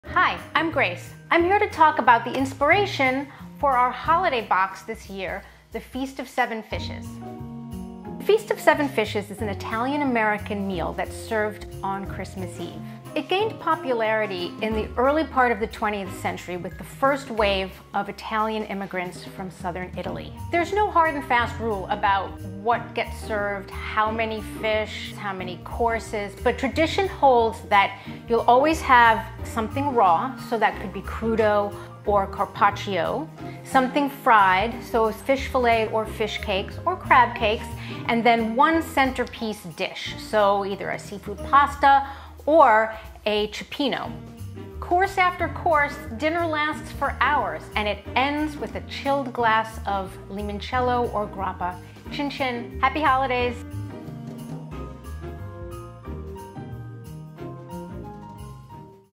The Feast of Seven Fishes | A Holiday Seafood Tradition
Curated by our Culinary Director, Grace Parisi, the Holiday Feast Box celebrates seven special seafood species in an ode to the traditional Italian Feast of Seven Fishes. Boasting nine pounds of wild-caught Alaska & North Pacific seafood and chef-developed recipes, our Holiday Feast Box makes meal planning quick and easy, giving you more time to spend with friends and family.

Sitka Salmon Shares is an online seafood market. Delivering wild and sustainable seafood to your door.